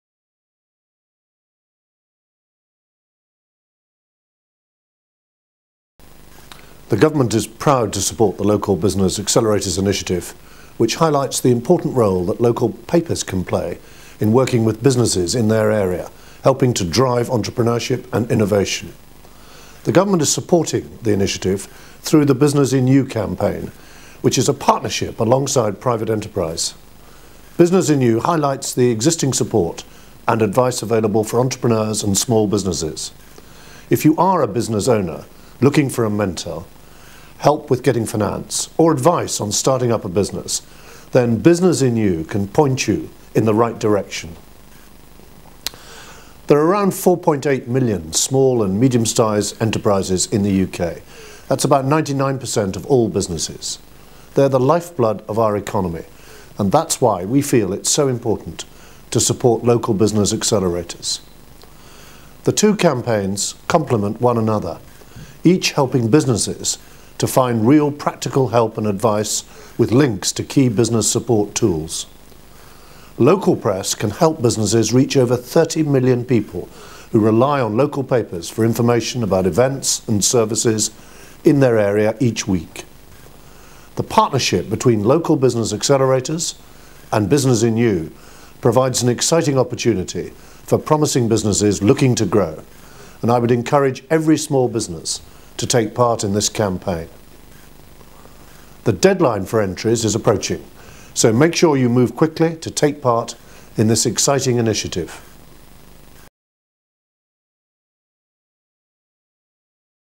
Rt Hon Michael Fallon MP
Rt Hon Michael Fallon MP on Local Business Accelerators and Business In You.